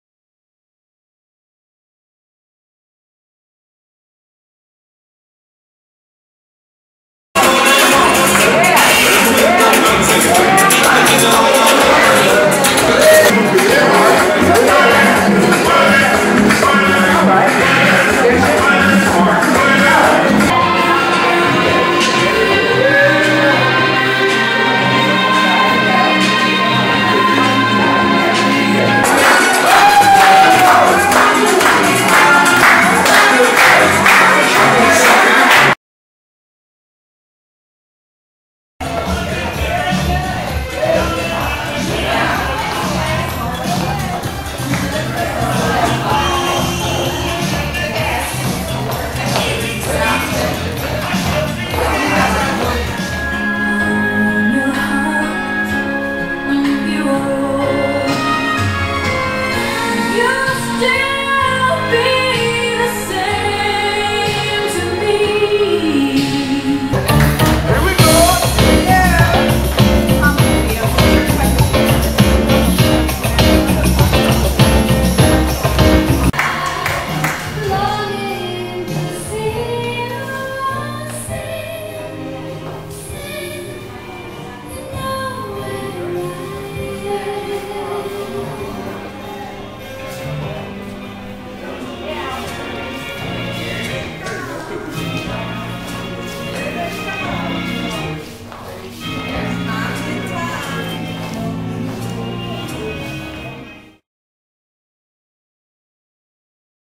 "Ballroom for the Brain" USA Dance program
Orlando Chapter of USA DANCE and the Alzheimer's Assn partnered to provide ballroom dance classes to early-stage Alzheimer's patients and their Care Partners. These video clips show USA DANCE volunteers guiding or partnering with the Alzheimer's participants, and the final class party, with refreshments, social dancing, and performances. Thank you to the Crosby YMCA for the beautiful dance room venue.